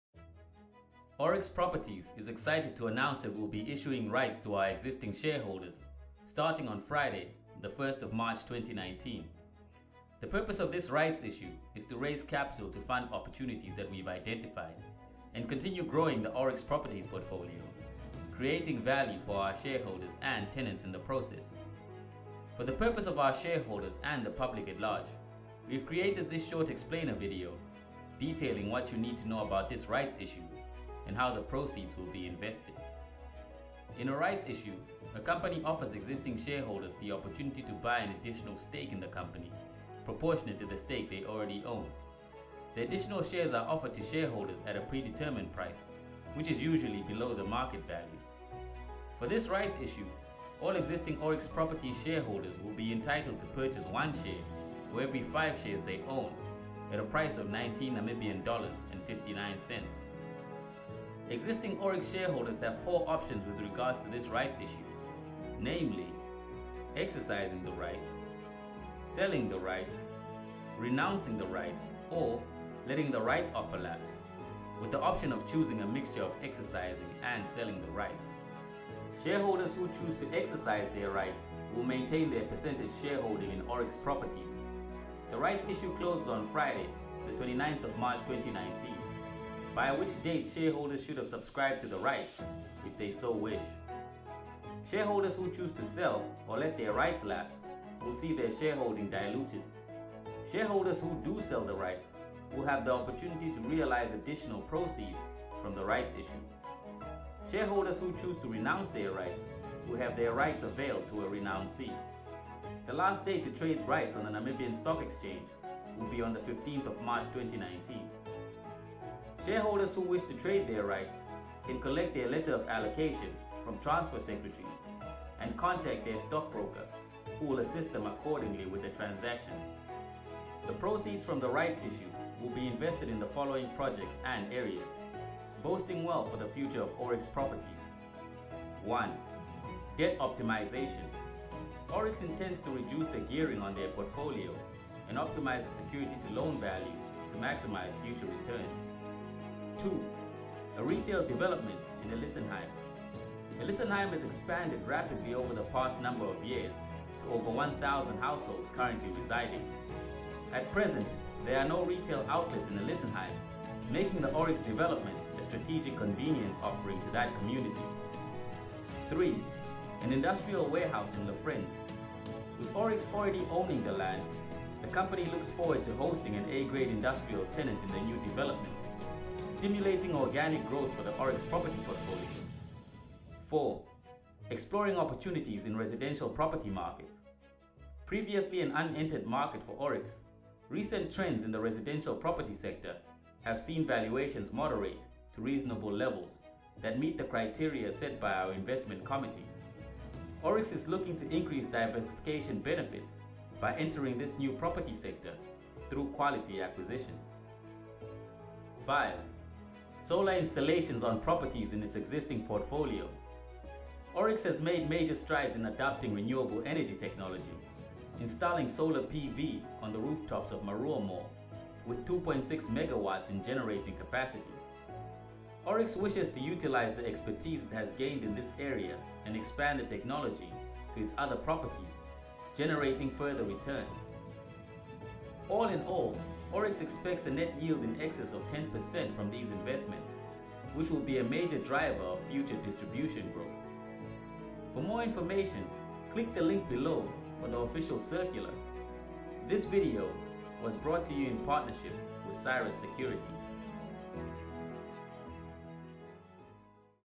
Oryx Properties: Rights Issue
Oryx Properties is in the process of doing a Rights Issue.

Shareholders have one of four options:
1. Exercise their rights
2. Trade their rights
3. Renounce their rights
4. Do nothing